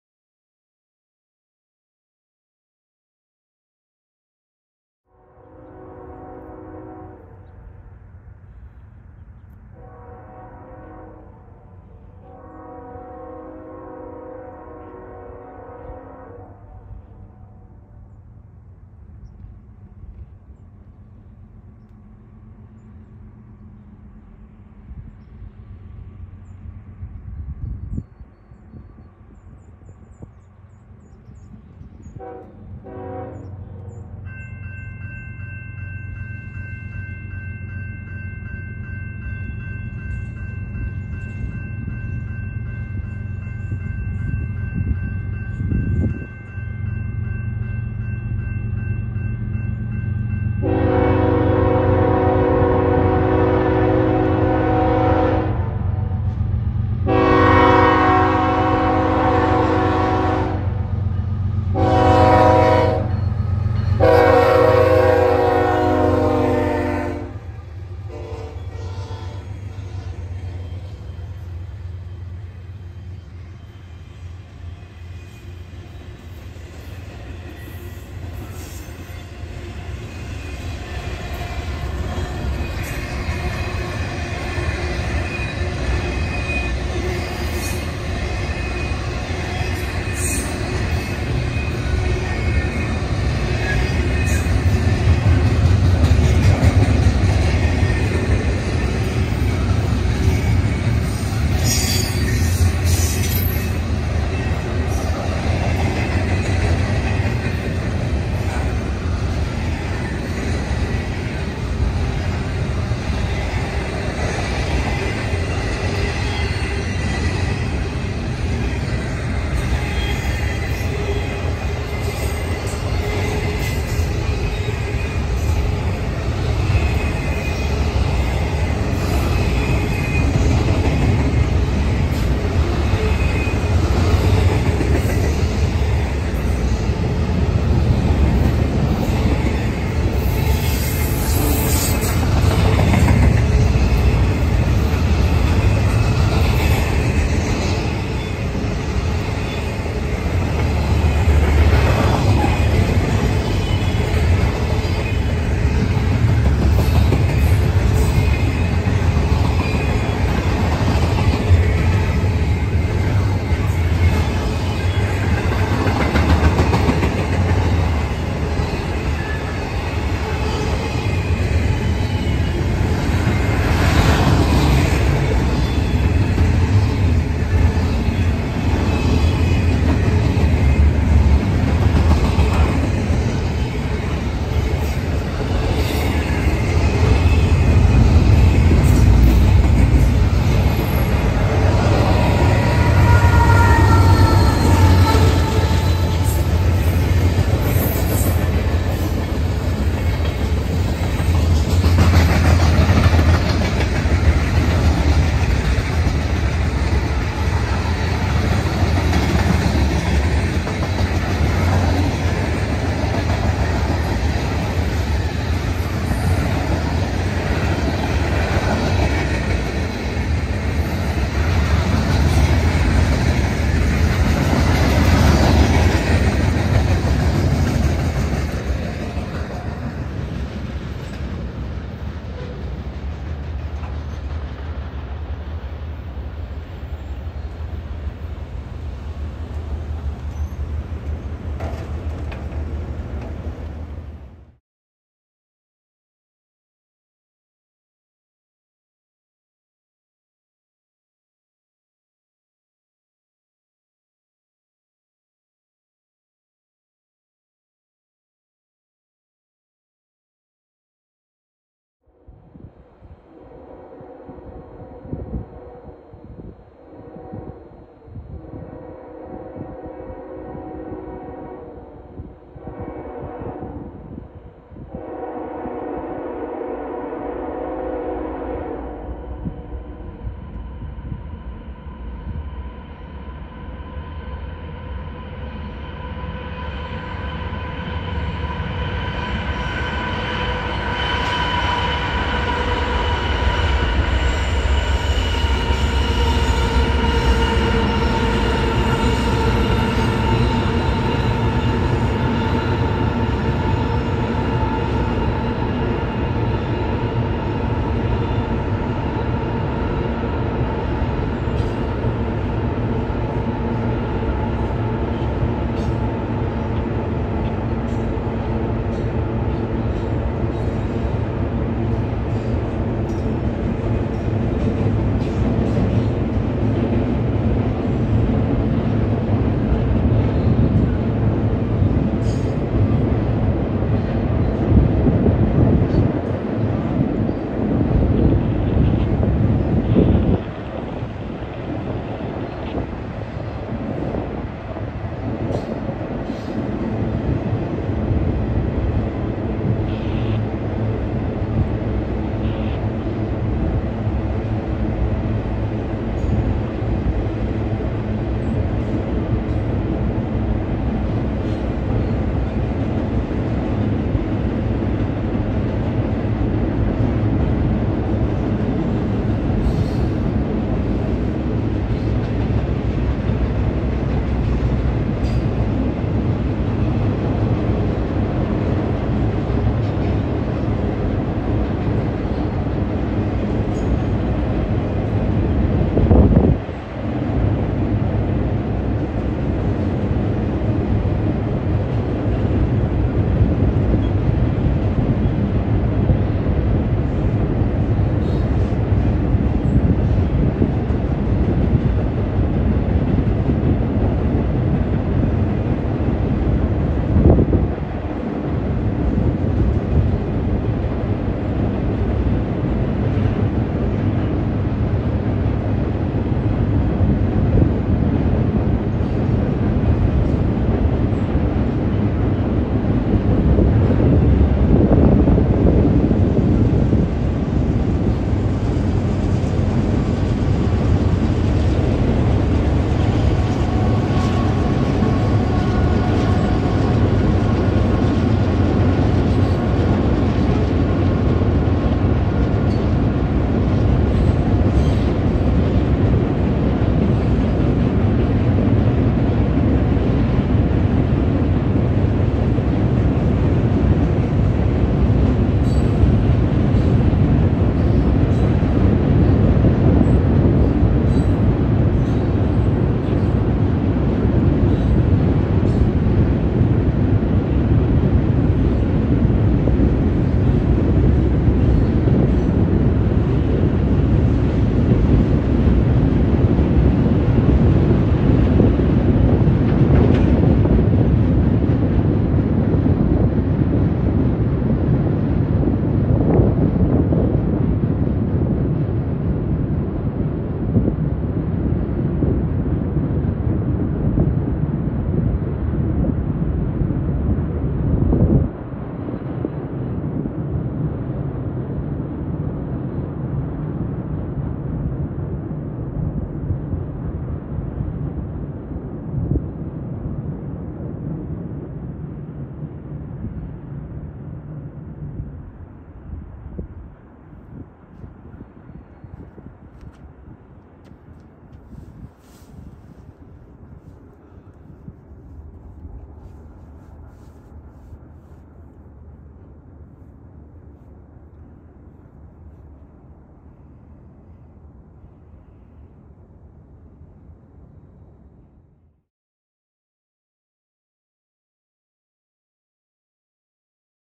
UP Leading NS’s 288 in Berryville, VA & Sheperdstown, WV
Berryville is a very nice town where I catch trains a lot. Shepherdstown is another nice town about 30 miles north of Berryville. The first steamboat was demonstrated in Shepherdstown, WV. on the Potomac River here.  James Rumsey invented the steamboat, demonstrated it on December 03, and Dec. 11th 1787. Also Colonel James Strode Swearinger was born here, he and his men he commanded, founded the city of Chicago in 1803.
I love this trestle over the Potomac River, it has been rebuilt and you still can see the old pilings up river only about 100 yards or so ( I’m just guessing about that). The height of the tracks to the water is approximately 100 feet. It’s a long way down. Standing at the monument for Rumsey is another height that is up there, you need to climb many steps, then the monument is sitting on more large marble structures.
Weather was chilly and mostly cloudy
Temperature 41°F or 5°C
Times 2:26pm and 4:15pm